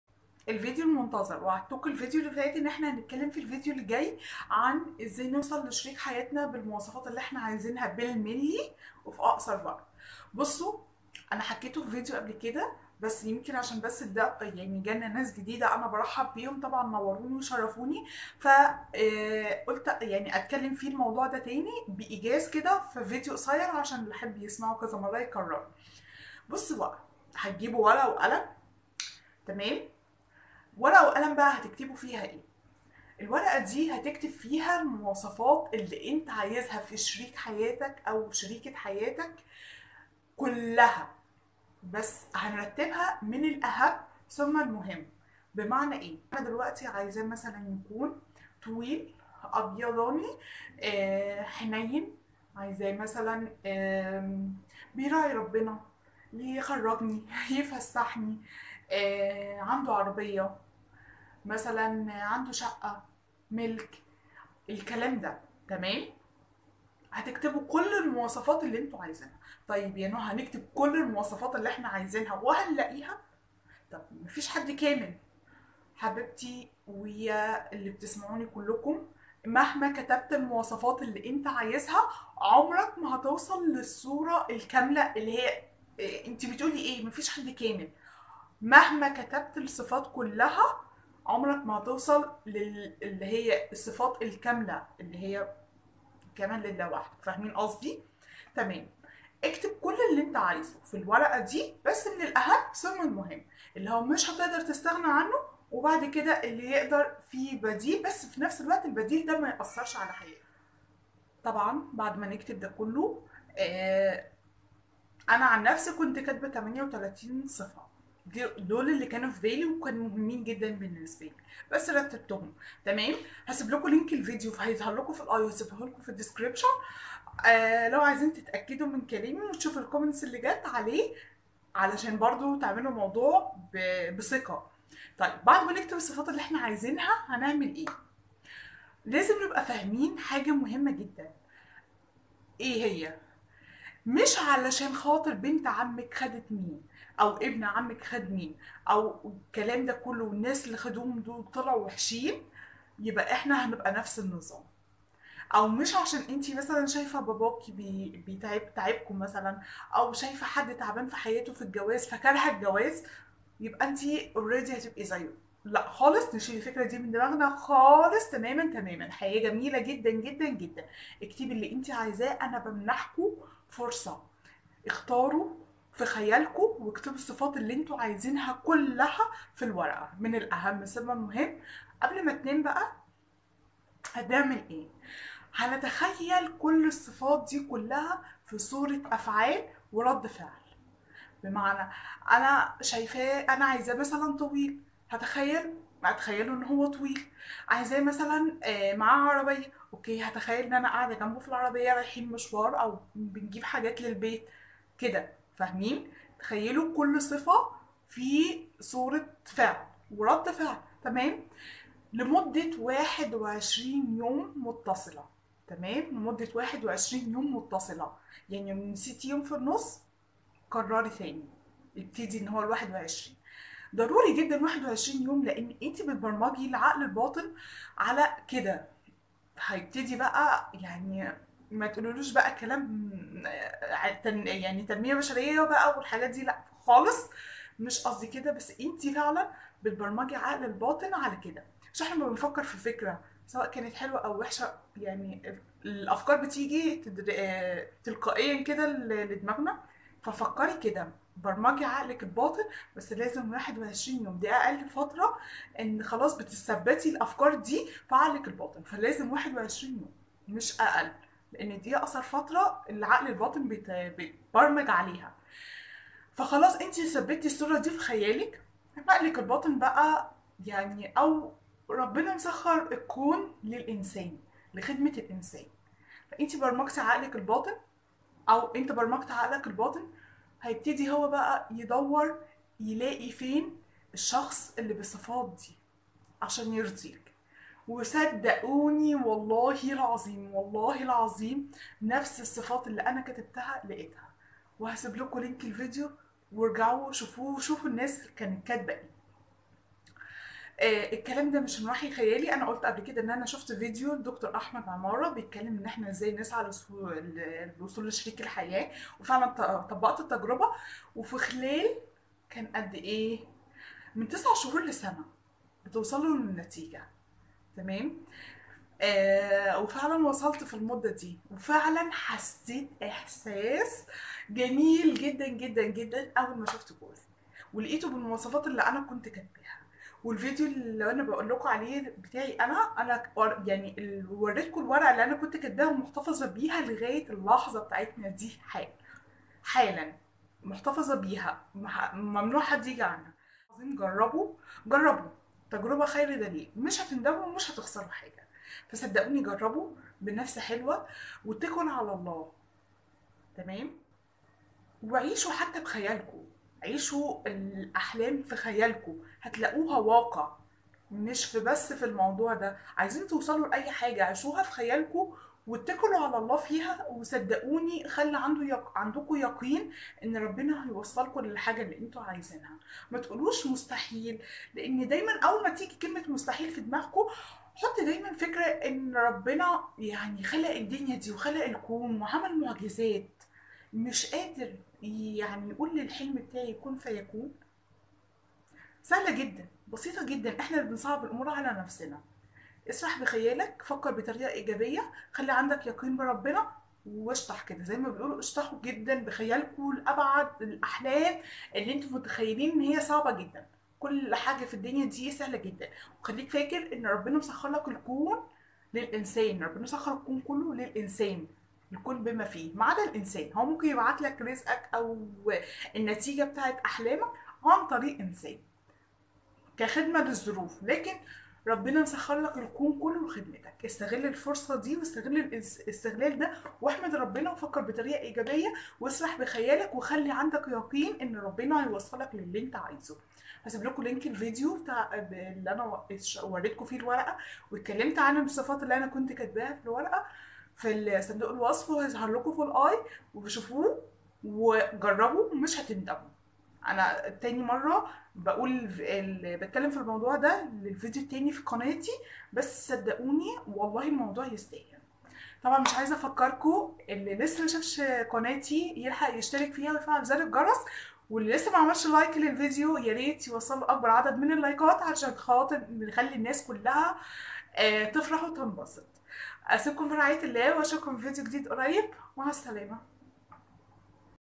لو قطر الجواز فاتك والعمر جري يبقي اسمع الفيديو ده هدية
حدد مواصفات شريك حياتك وهتلاقيها كلها ومهما كان عمرك خلاصة تجربتي

لازم ندور ونسعي لشريك حياتنا وهتلاقيه بكل المواصفات الي بنتمناها